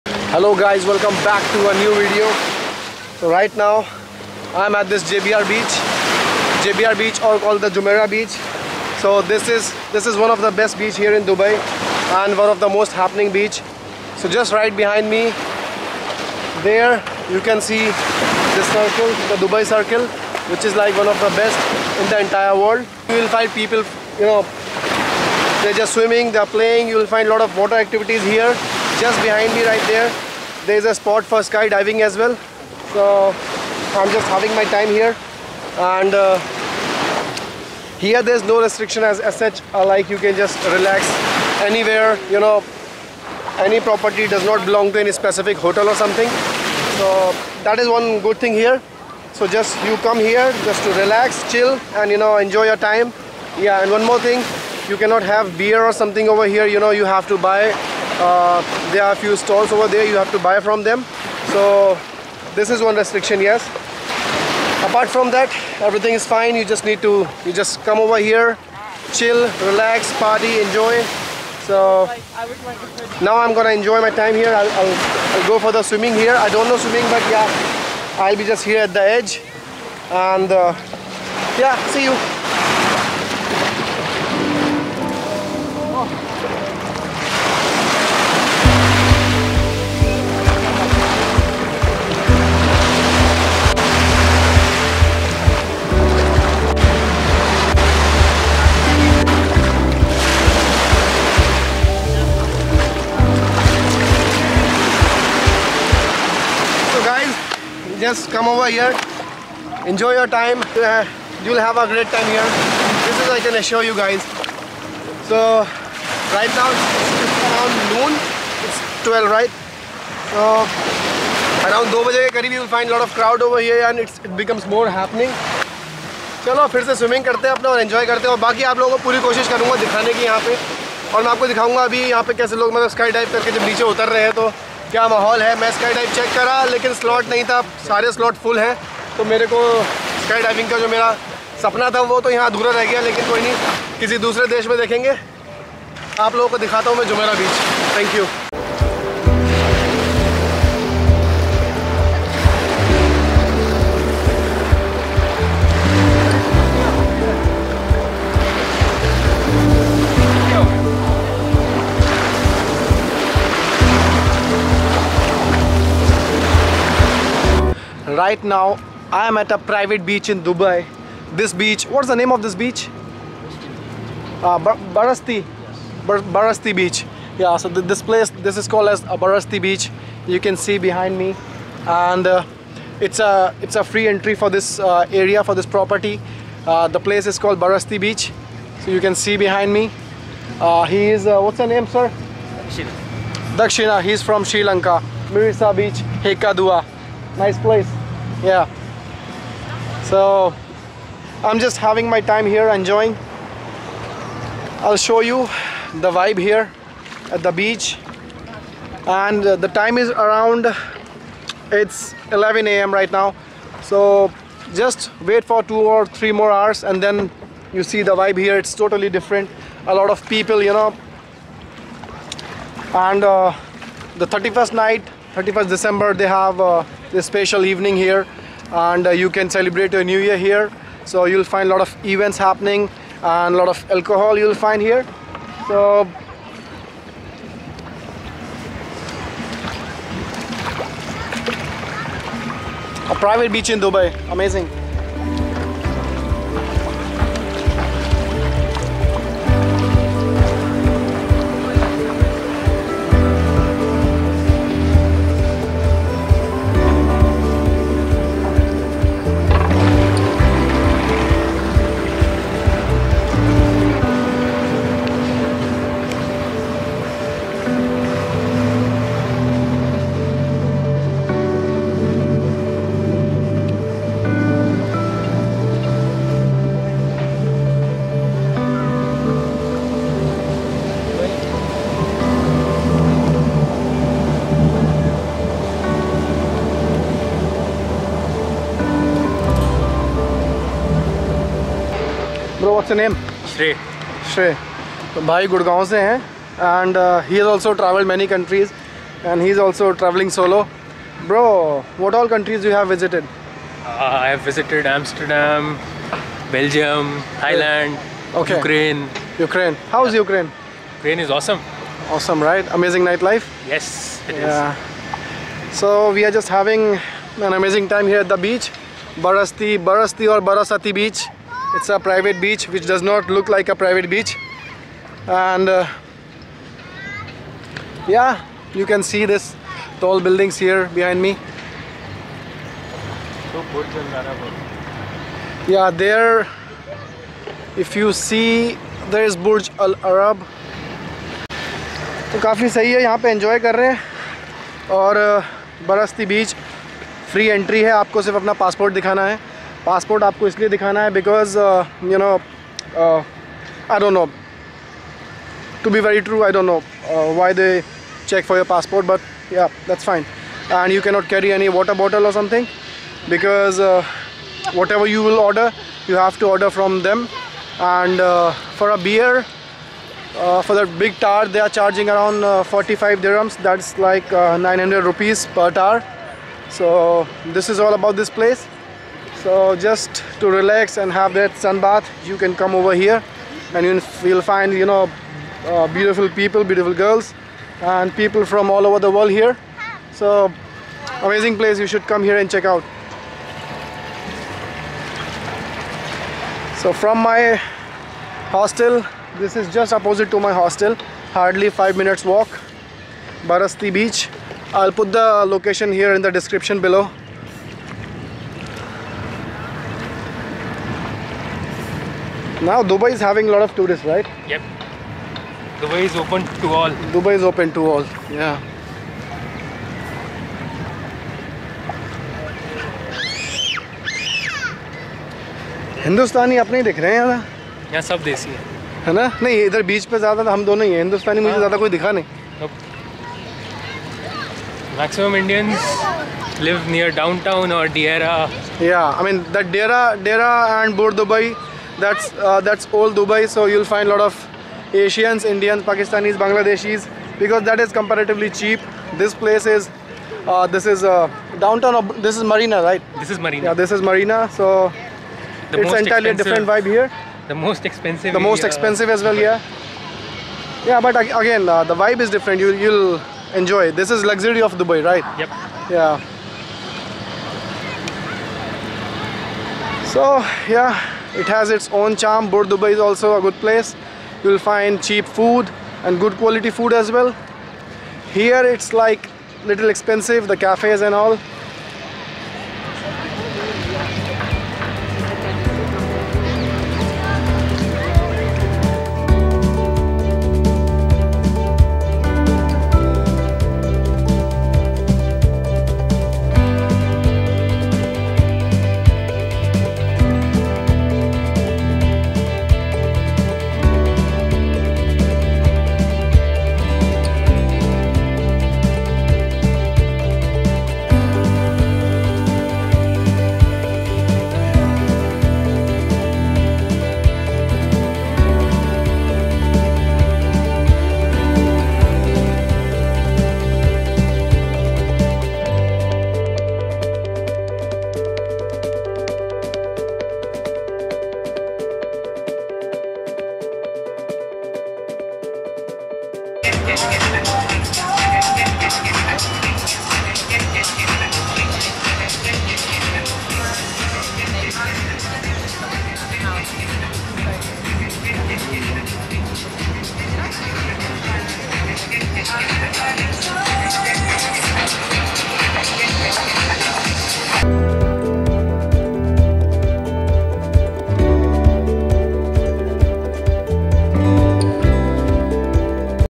JBR Beach, Jumeirah Beach || Best Beach In Dubai || Barasti Beach- Private Beach In Dubai
In this video, I am gonna show you the best beach in Dubai - Jumeirah Beach aka JBR Beach.

Along with that I will show you a private beach in Dubai which is “Barasti Beach” !! You need to show your passport to get a free entry to this Beach. It’s located in Dubai Marina just opposite to JW MARRIOTT HOTEL.

Gadgets used :

1- iPhone XS MAX

2- DJI OSMO MOBILE 3

3- GO PRO HERO 7 BLACK